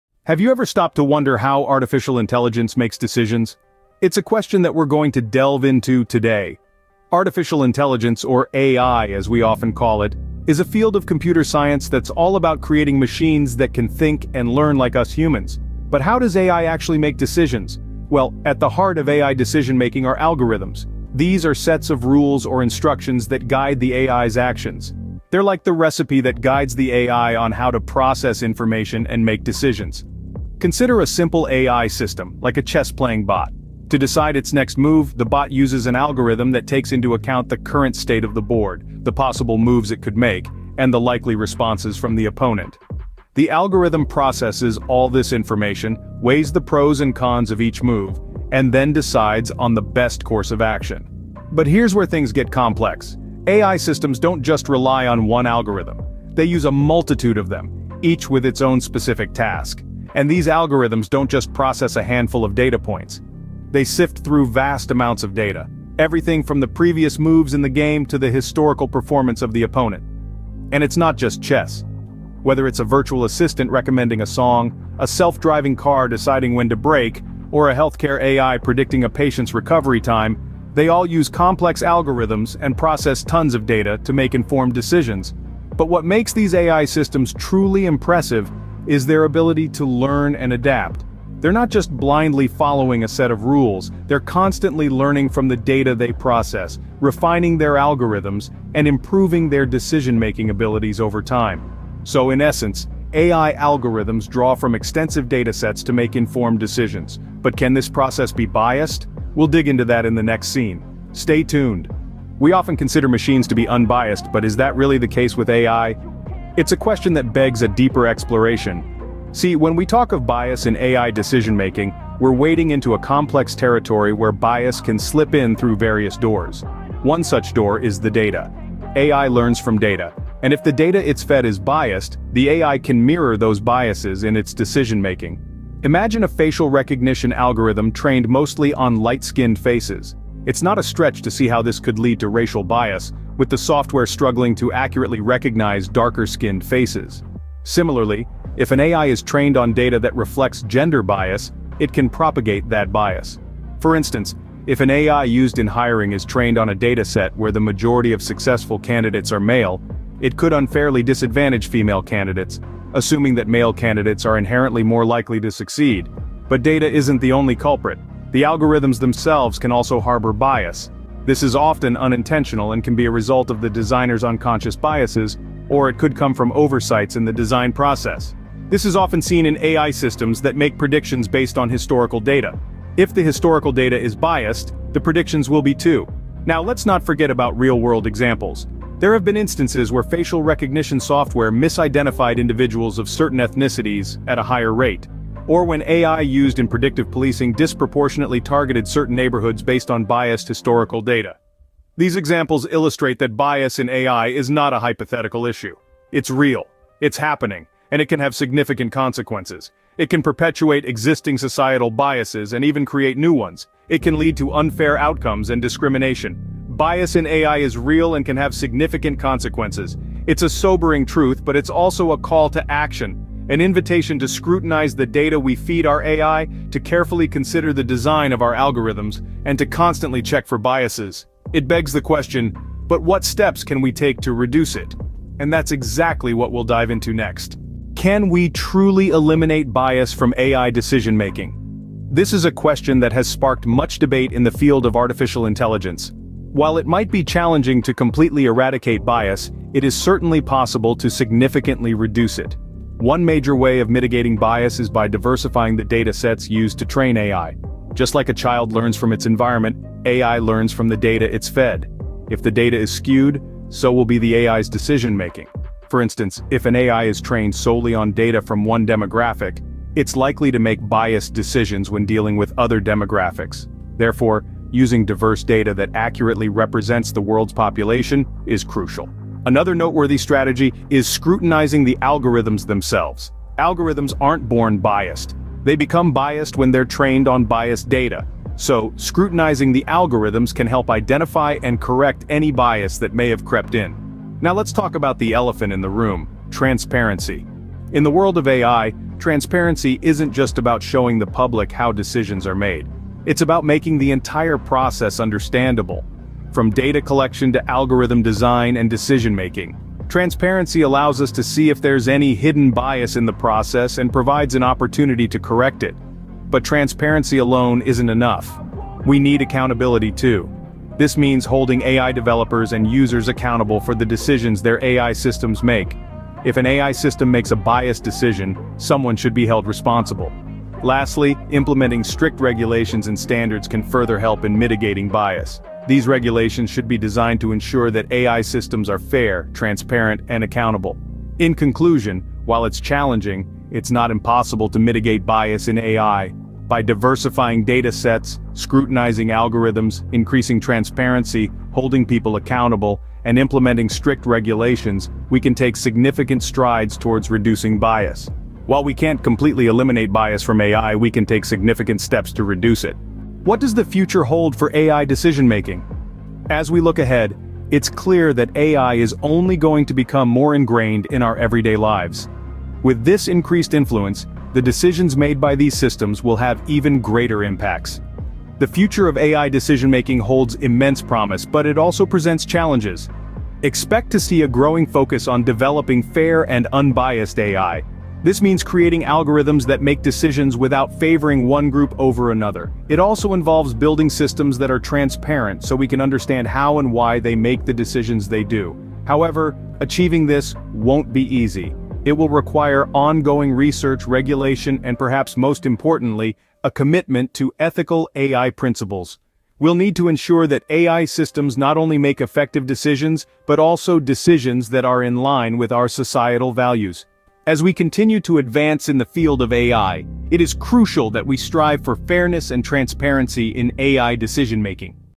Decoding AI: Unveiling the Bias in Algorithms
Video Description:
In this thought-provoking video, we delve into the fascinating world of artificial intelligence (AI). Join us as we demystify the decision-making process within AI systems, shedding light on how algorithms make choices and the potential for bias. We explore real-world examples and discuss the implications of biased AI. This eye-opening discussion is essential for anyone interested in technology, ethics, and the future of AI. Let's spread awareness together.

OUTLINE:

00:00:00 Unraveling AI Decision-making
00:01:52 The Bias in AI Decision-making
00:03:57 Mitigating Bias in AI
00:06:03 The Future of AI Decision-making